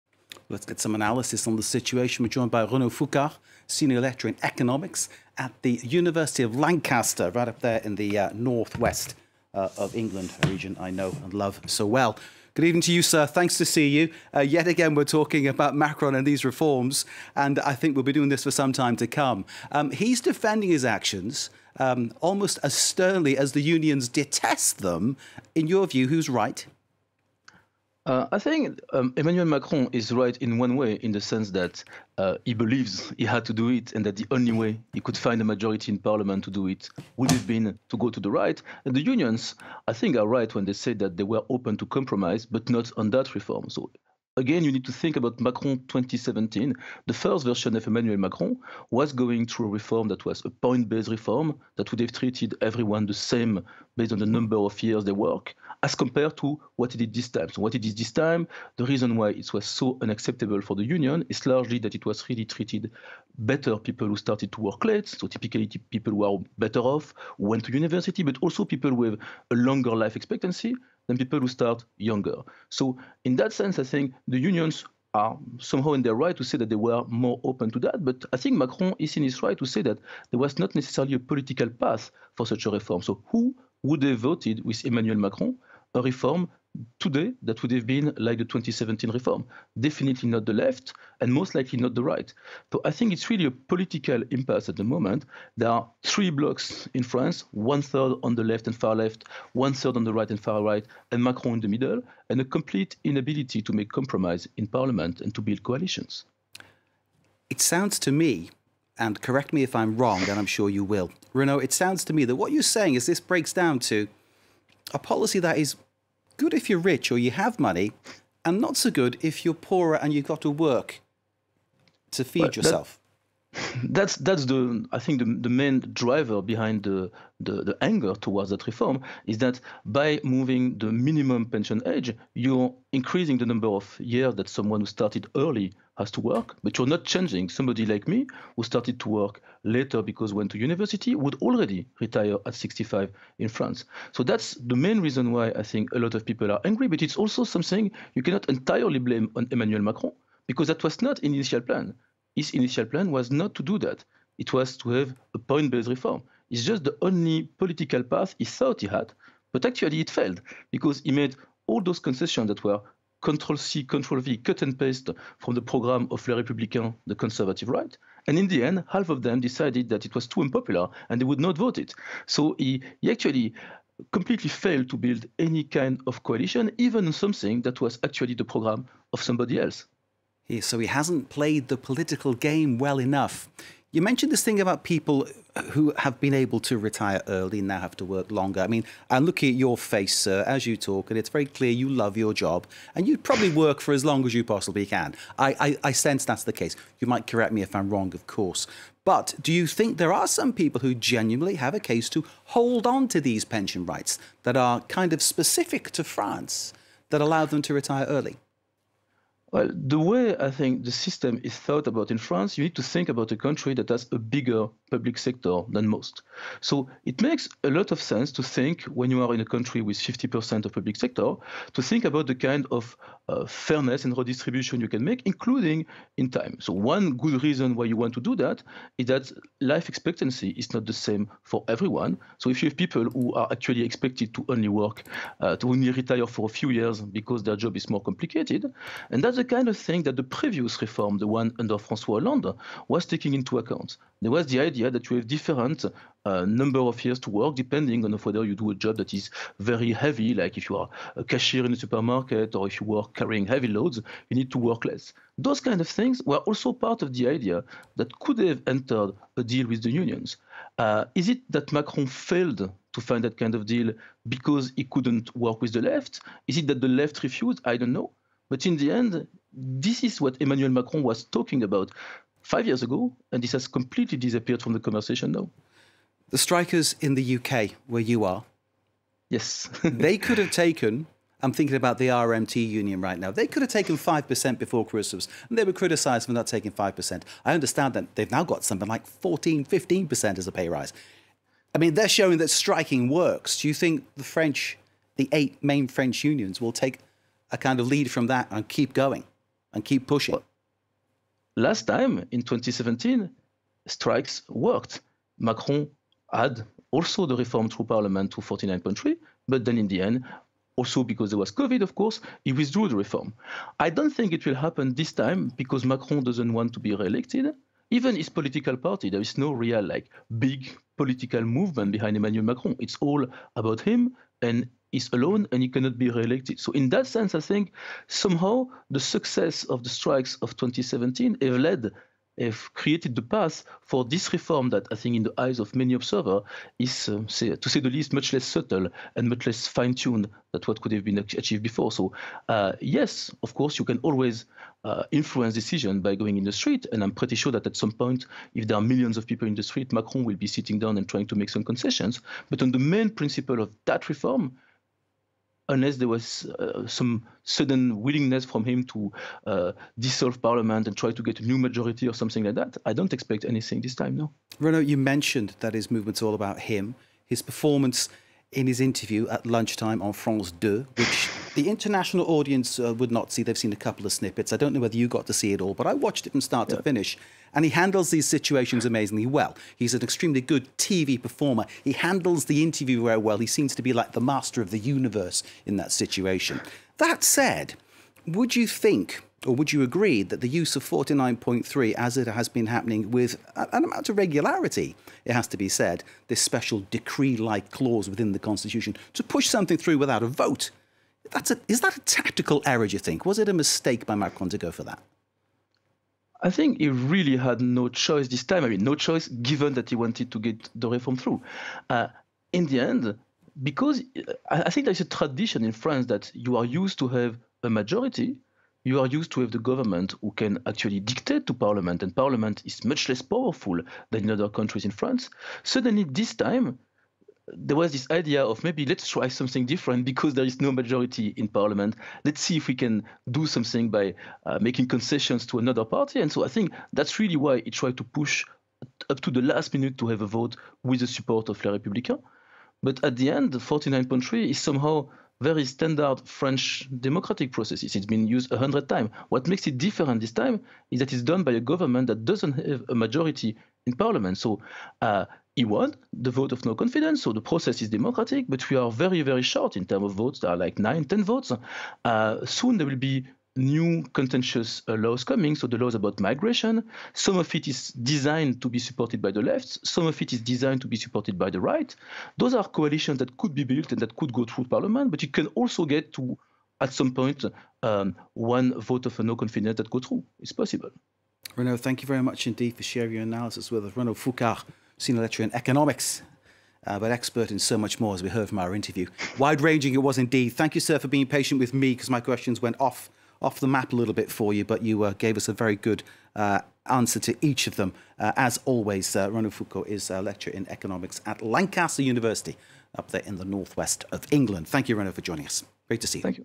Macron embraced Republicans' pension compromise, 'completely failed to build any kind of coalition'
French President Emmanuel Macron on Wednesday defiantly vowed to push through a controversial pensions reform, saying he was prepared to accept unpopularity in the face of sometimes violent protests. Prime Minister Elisabeth Borne, acting on the president's instructions, last Thursday invoked an article in the constitution that adopted the contentious reform without a parliamentary vote. For more on the pension reform showdown and the ensuing fallout, FRANCE 24 is joined by Dr. Renaud Foucart, Senior Lecturer at the Department of Economics at Lancaster University Management School.